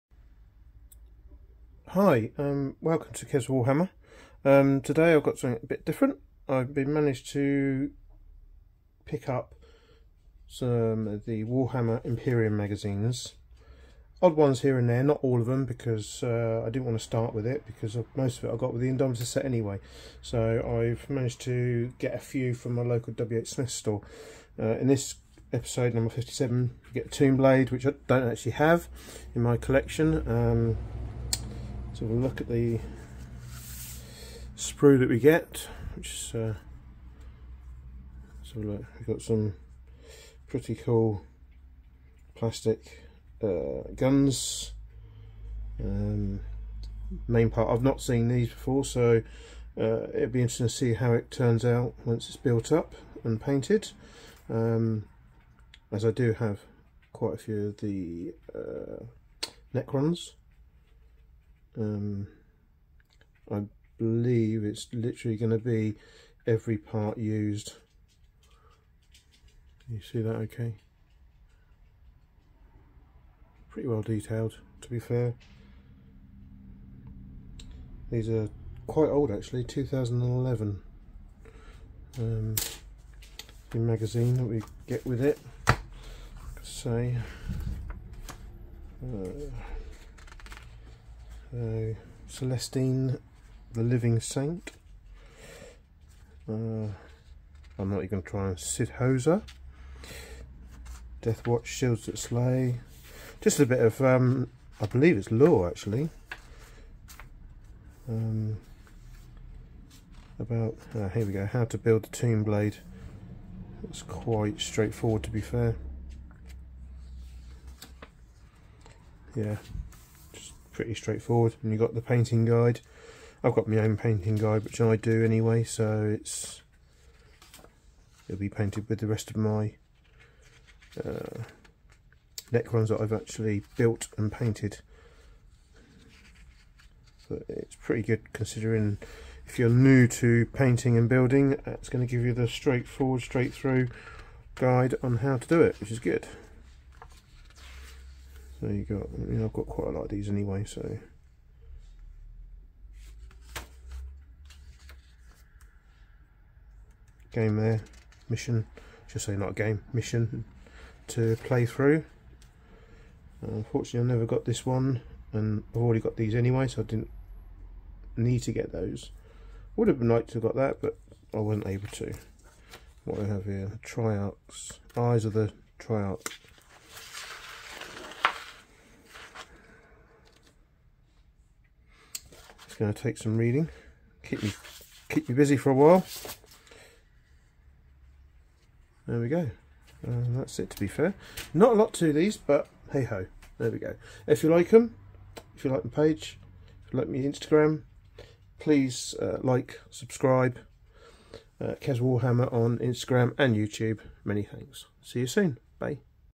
warhammer imperium magazine
my review of the warhammer imperium partworks magazine